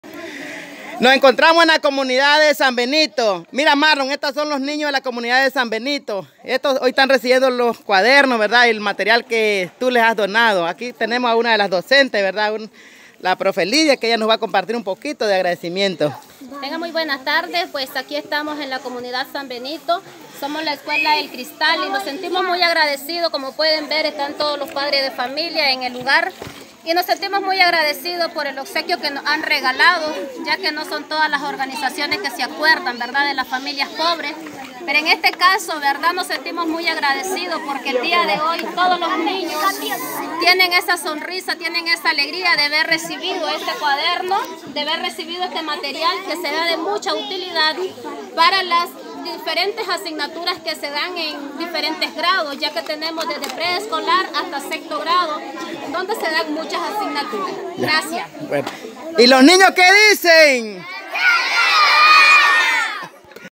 Nicaragua Travel
Nicaragua travel in 2020. Children receiving school supplies.